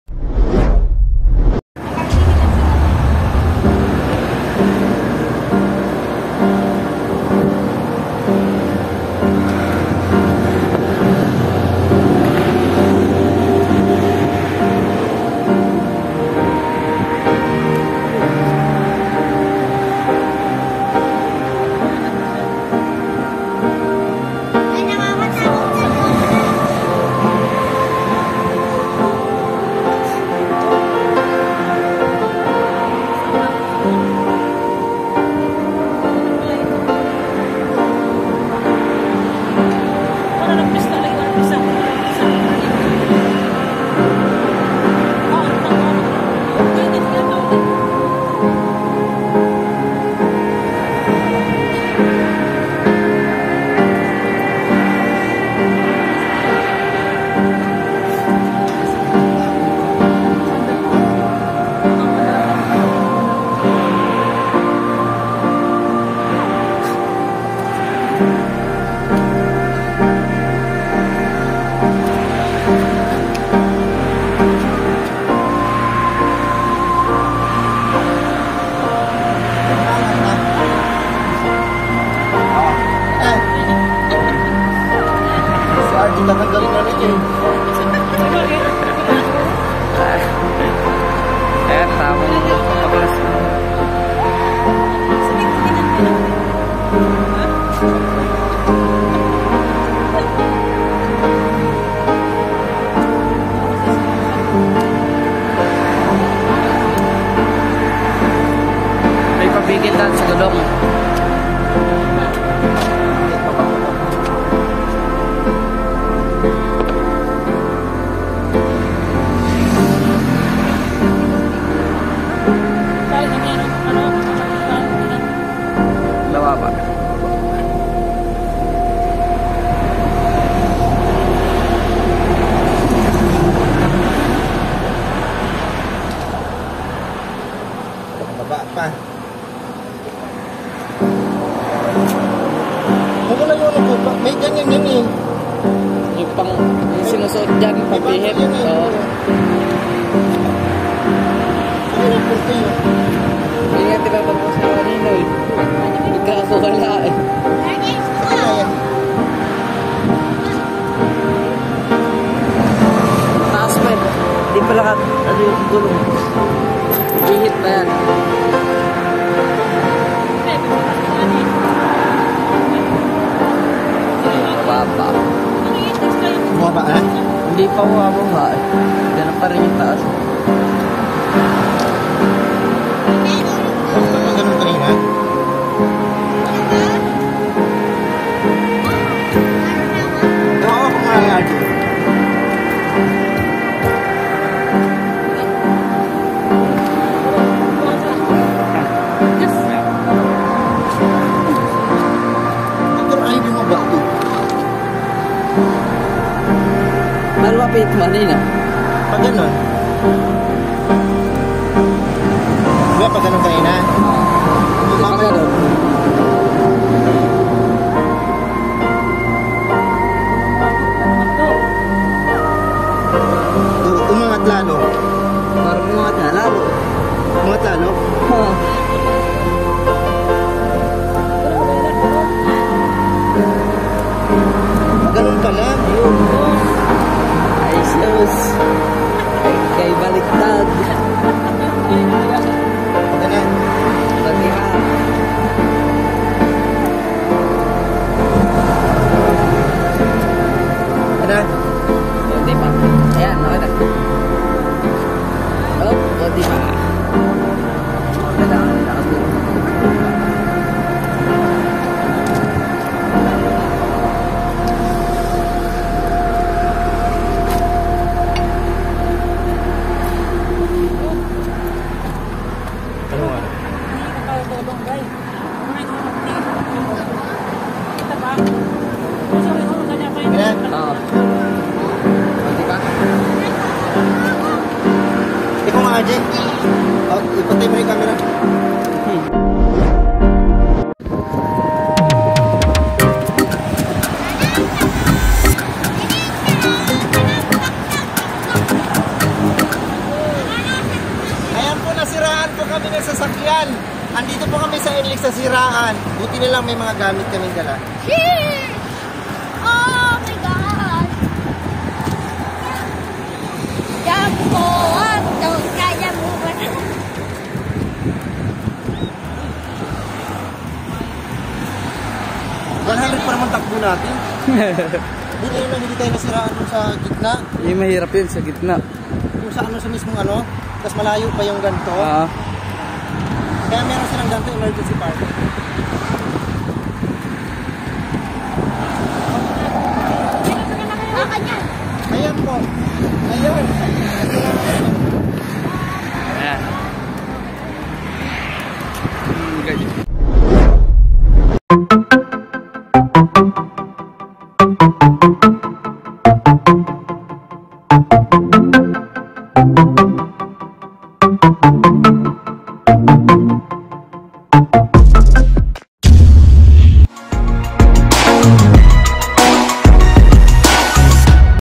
FIRST TIME MASIRAAN SA DAAN SA NLEX PA PUMUTOK YUNG GULONG NG COTSE KO
Grabee kaba ko first time nasiraan sa daan sa NLEX pa pumutok yung gulong ng cotse ko